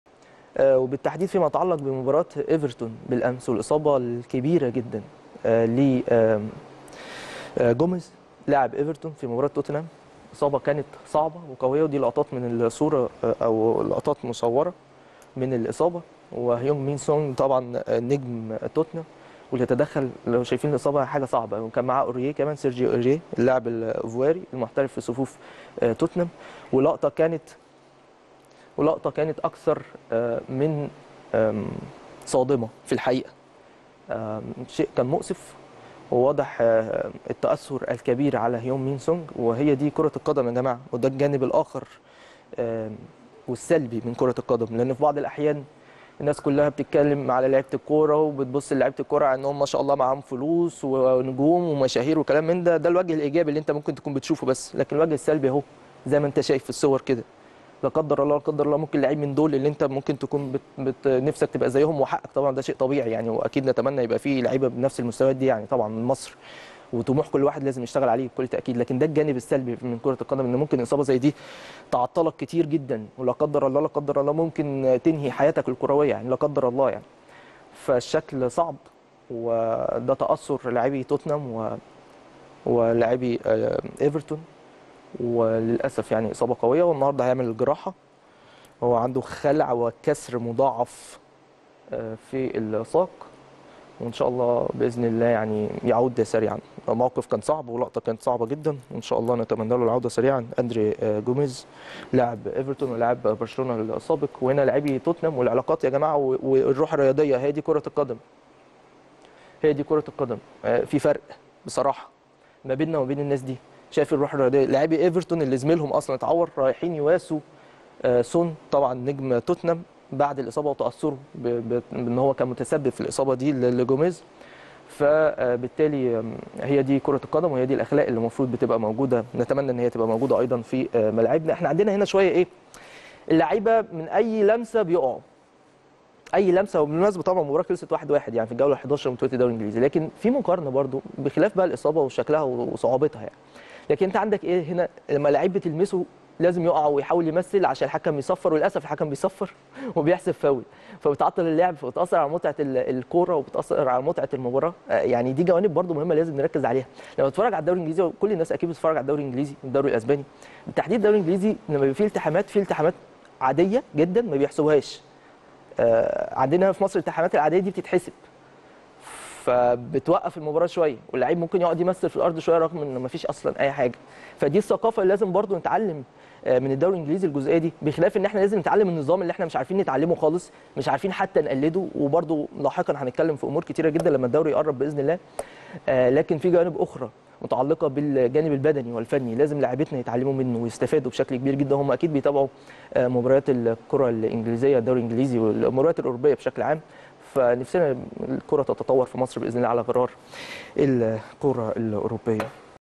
أمير هشام يستعرض آخر تطورات إصابة جوميز الخطيرة
التردد: 12341 أفقي.
القمر: نايل سات.
معامل الخطأ: 5/6.
معدل الترميز: 27500